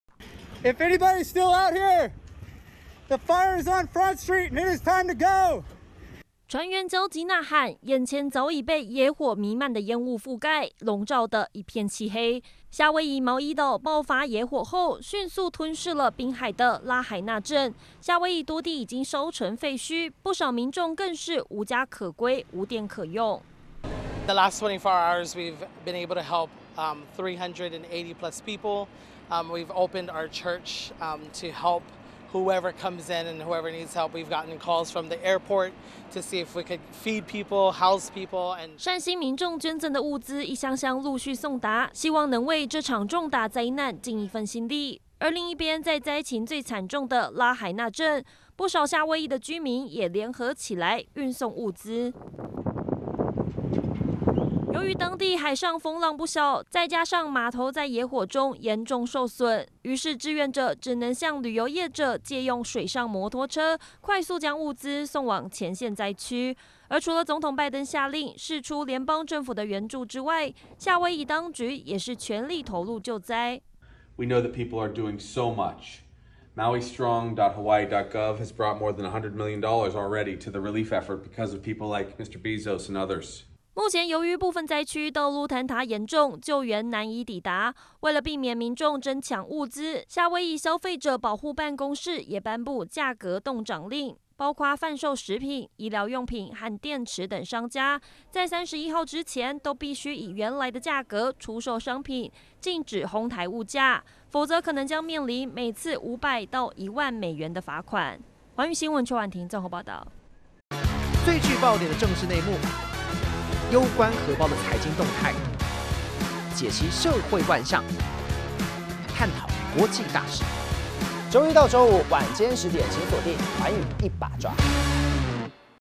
野火重創夏威夷毛伊島 物資極缺 民間自發救援｜寰宇新聞
美國夏威夷州毛伊島遭到野火重創，超過1千棟建築 被燒成廢墟，許多民眾不只無家可歸，更在嚴重 缺乏物資的情況下，苦苦等待救援。在災情最慘重的 拉海納鎮，甚至有 志願者 借來水上摩托車，運送物資。而夏威夷當局 也頒布「凍漲令」，嚴格禁止哄抬物價的行為。